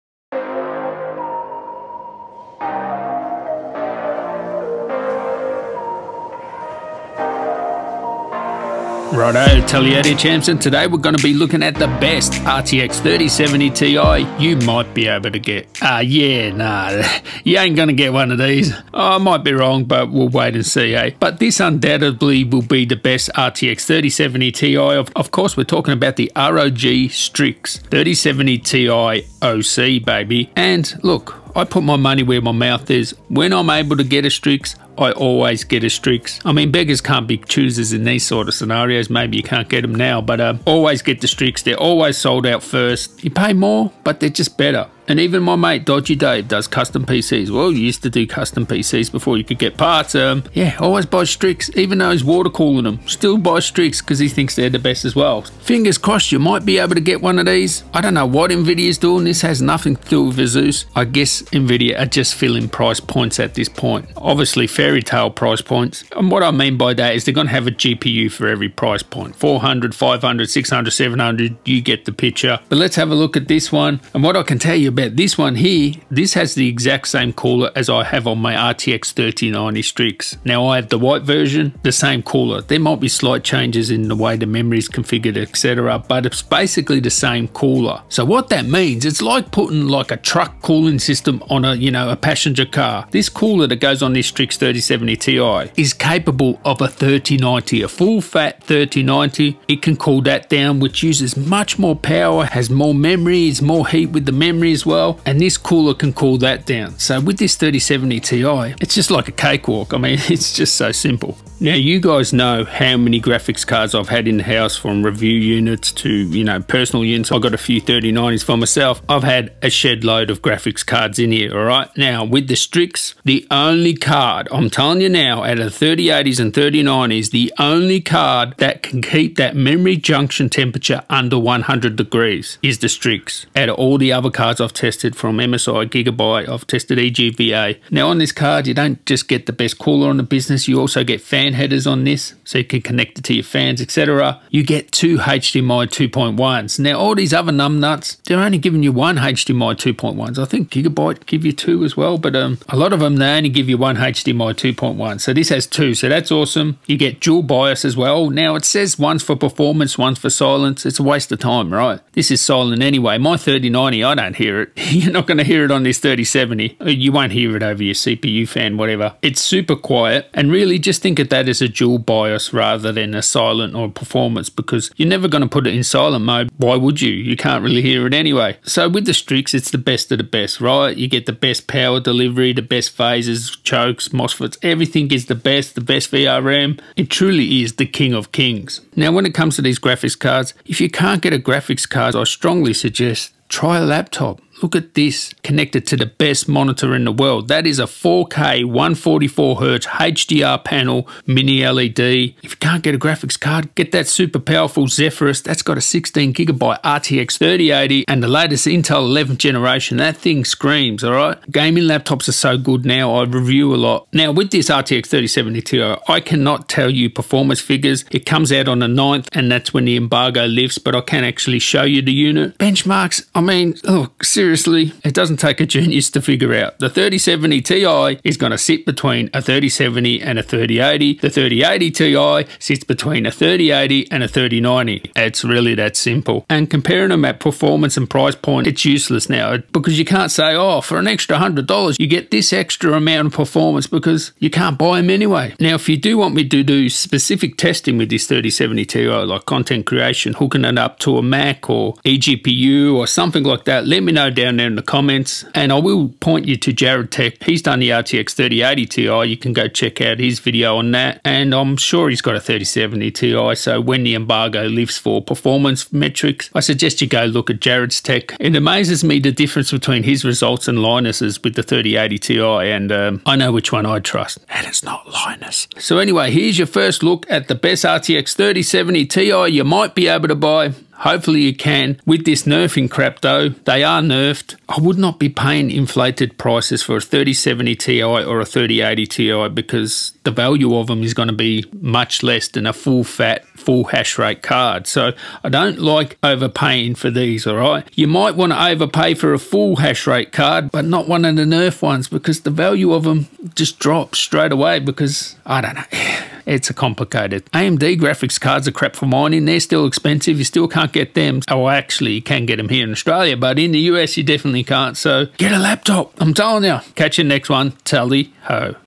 RTX 3070 Ti ROG STRIX OC 8GB Unboxing & First Look The Best RTX 3070 Ti You Might Be Able to Buy 🤌
The Best RTX 3070 Ti You Might Be Able to Buy - ROG STRIX RTX 3070 Ti OC unboxing and first look review.

⏩ Kaspersky Total Security 1 Device 12 Months PC Digital Code GlOBAL $26.36

⏩ Recommended SSD's for Laptops: